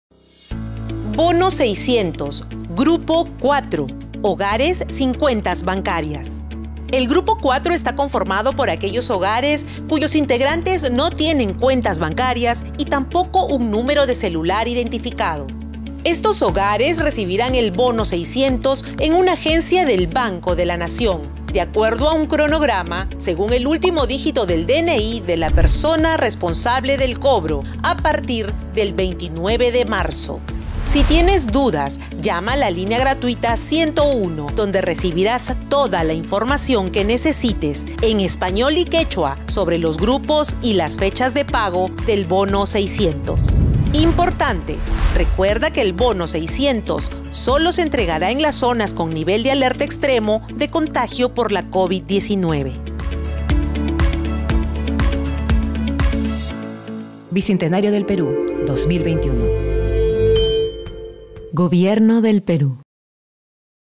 Bono600 | Grupo 4
🗓️ A partir de este lunes 29 de marzo, los hogares sin cuentas bancarias recibirán el Bono600.
▶️ Entérate más en este video.